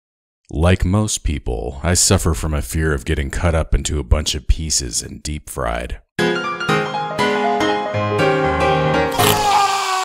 Potato fears
Like most people, I suffer from the fear of getting cut up into a bunch of pieces and deep fried.